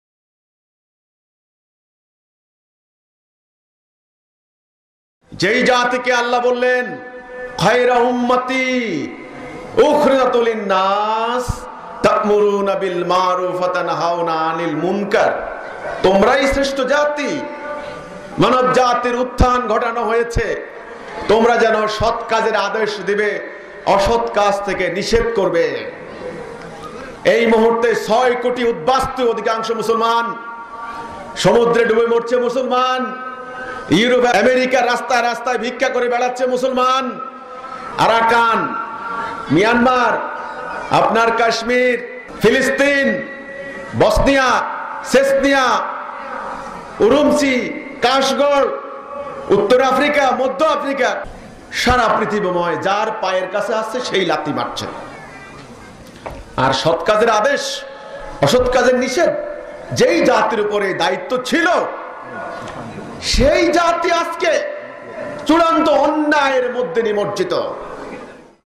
মুসলিম জাতির অবস্থা
২৩ জানুয়ারি ২০১৭
চয়াডাঙ্গা মোমেনপুর শরিষাডাঙ্গা বাউল একাডেমি মাঠে সন্ত্রাস, জঙ্গিবাদ ও সাম্প্রদায়িকতার বিরুদ্ধে শীতকালিন ধর্ম সভায় মাননীয় এমামের বক্তব্যর খন্ডচিত্র।